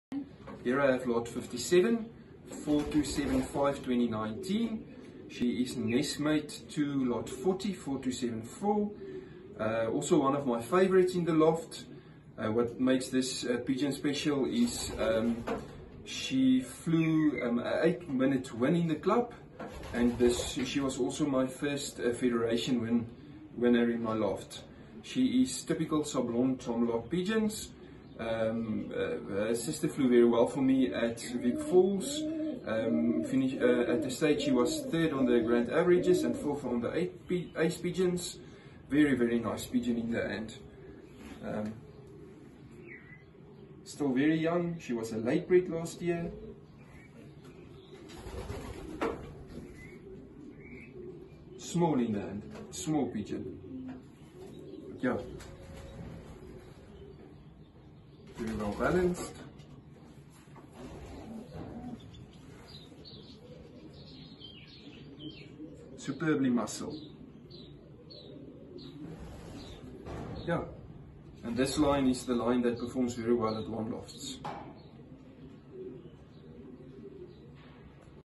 LOT 57 - Danie Scholtz - ZA HPT 4275 19 HEN - Auction by
Sire, ZA BPF 1026 ’18 bred for stock, his father “Rockey” bred OLR winners.  His full brothers and sisters bred union, federation and OLR winners!  His mother bred double equal OLR winner, her full brother and sisters bred union, federation and OLR winners. Dam, ZA 10160 ’16, super bred from Tom Lock’s best.  R100 000 Egg cock, Witoog hen, R80 000 Super cock and his click hen, SA THU 1648 ’03 all in her pedigree. ZA HPT 4275 ’19, together with her brother, ZA HPT 4274 ’19, some of the best handlers in my loft.  Small to medium sized cock, well balanced, muscled.  Full brother to ZA HPT 3527 ’19 “Maite” who raced consistently at Victoria Falls World Challenge Pigeon Race 2020. Performances:  1, 29 Please see additional info for all positions flown. What makes this win very special to me, is that ZA HPT 4275 ’19 won the club with almost 08 minutes, as well as my first Federation win ever!! SIRE: Alias "ROCKY'S GOLD" Strain: 810 SABLON COLLECTION Color: Blue Bar Bred for stock, his father "Rocky" bred OLR winner. His full brothers and sisters bred union, federation, OLR winners ! His mother bred double equal OLR winner, her full brother and sisters bred union, federation and OLR winners. DAM: Strain: R100 000 X Witoog Hen Color: Blue Bar White Flt A very pretty little hen. Super bred pigeon from Tom Lock's best.